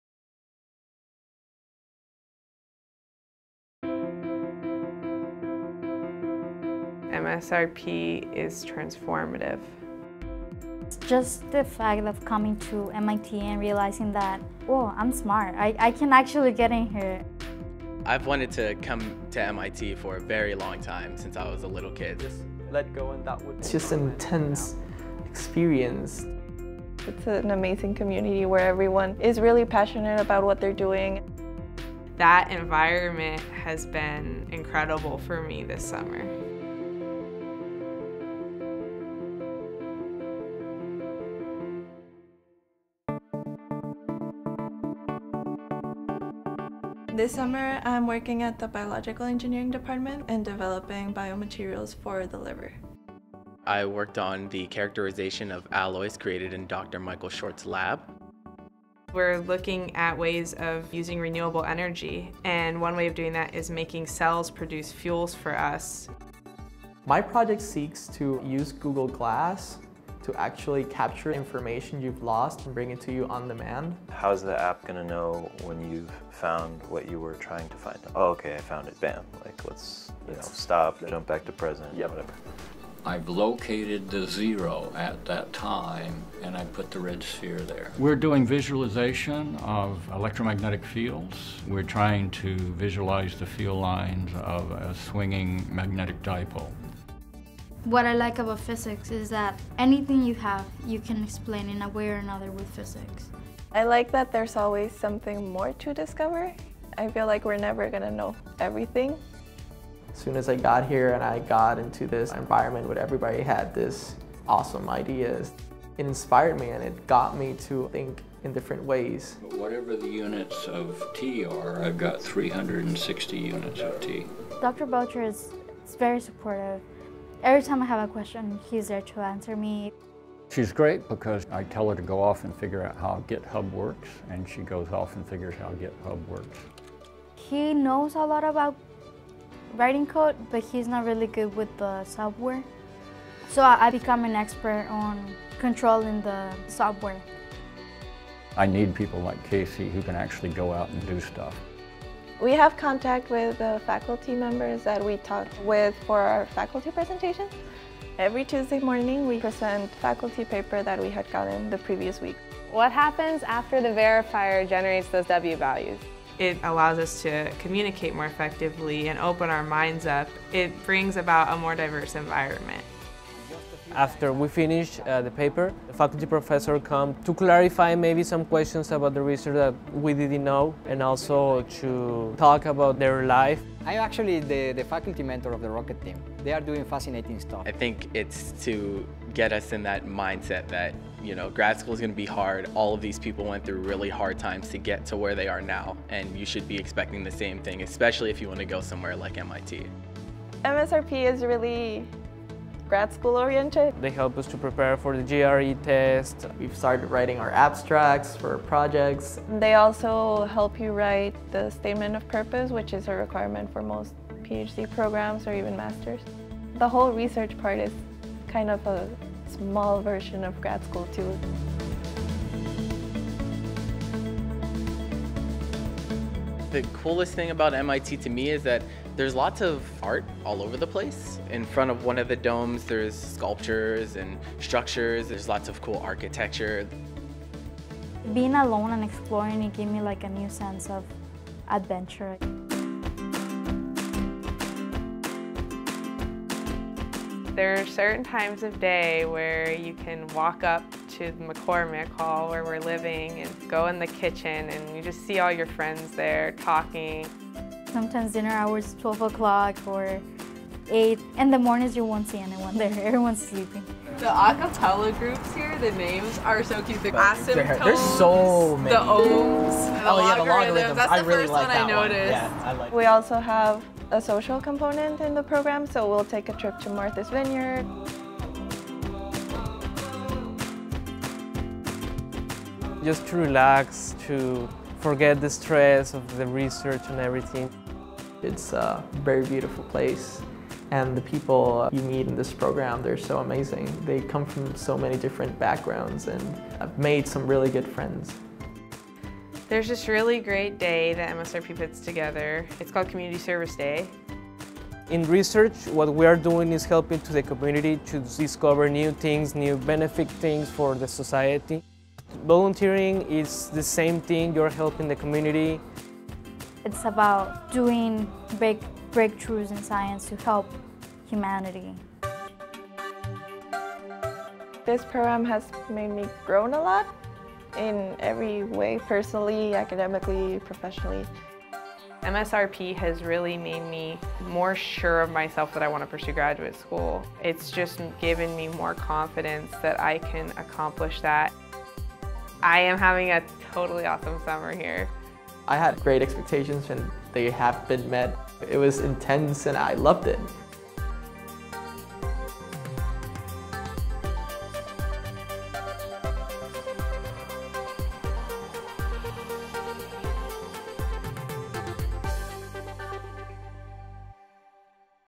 MIT Summer Research Program
Undergraduates from universities around the country are recruited to spend the summer conducting research on the MIT campus while preparing for the transition to Graduate School.